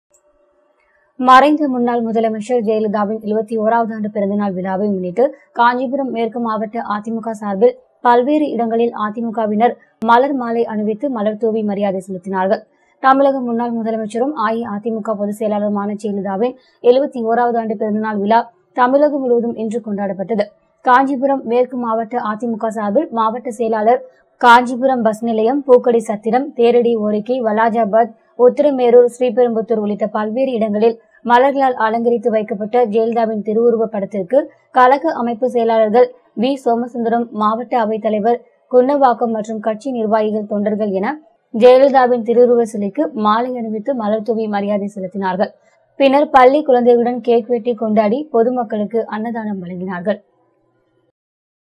|KANCHIPURAM|கேக் வெட்டி அன்னதானம் வழங்கி கொண்டாடிய அதிமுக தொண்டர்கள் |
|Jayalalitha birthday in KANCHIPURAM|
For more updates: